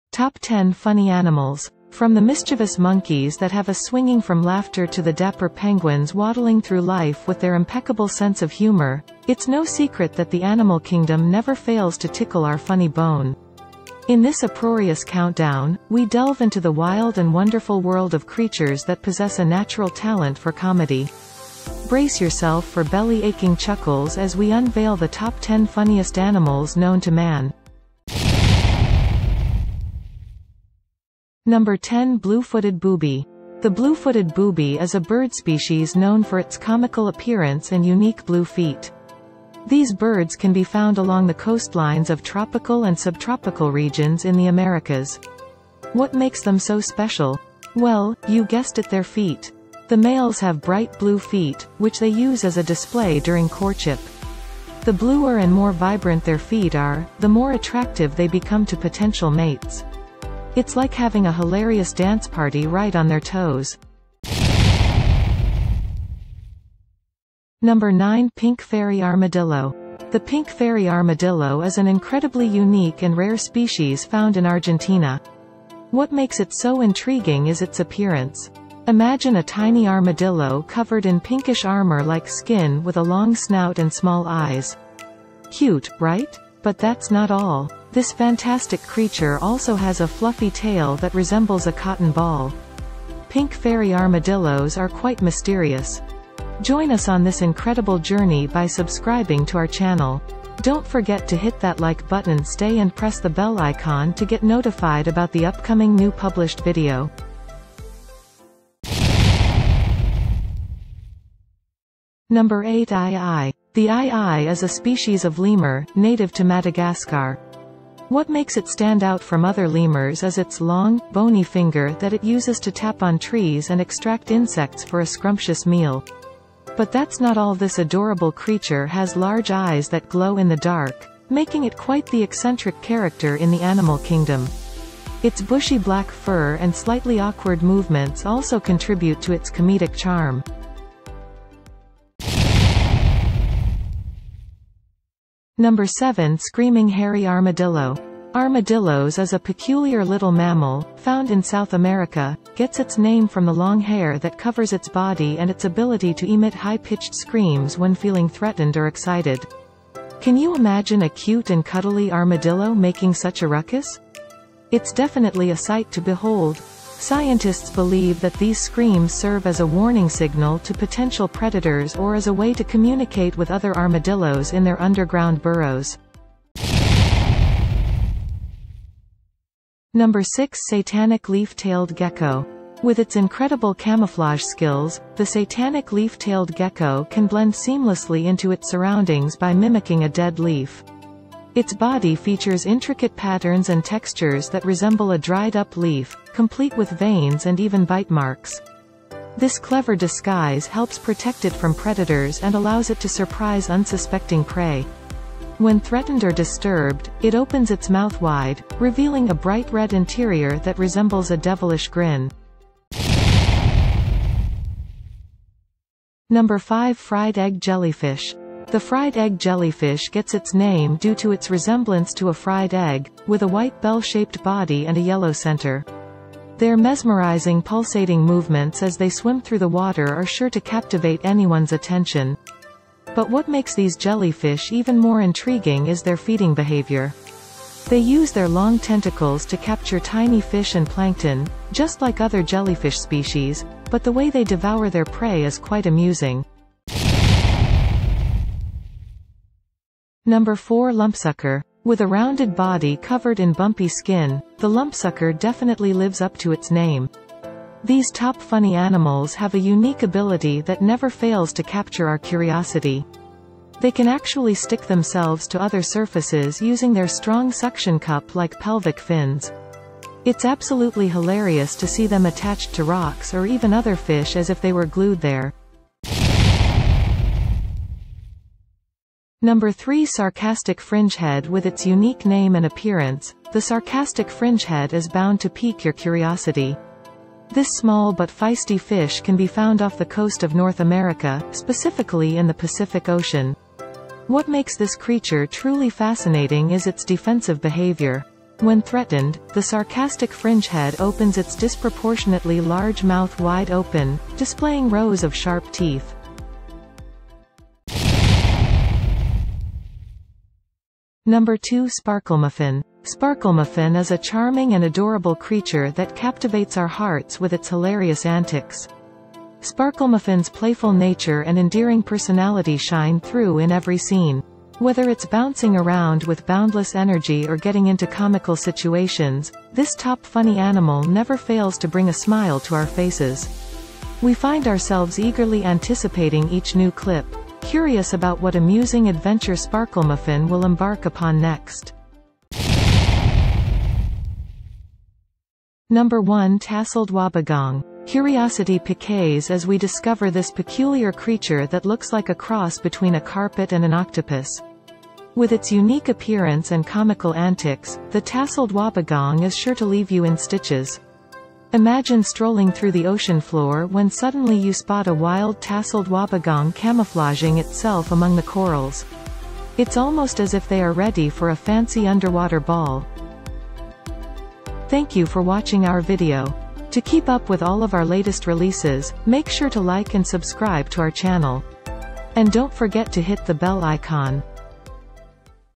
Explore the top 10 funny animals
From the mischievous monkeys that have us swinging from laughter to the dapper penguins waddling through life with their impeccable sense of humor, it's no secret that the animal kingdom never fails to tickle our funny bone. In this uproarious countdown, we delve into the wild and wonderful world of creatures that possess a natural talent for comedy. Brace yourself for belly-aching chuckles as we unveil the top 10 funniest animals known to man.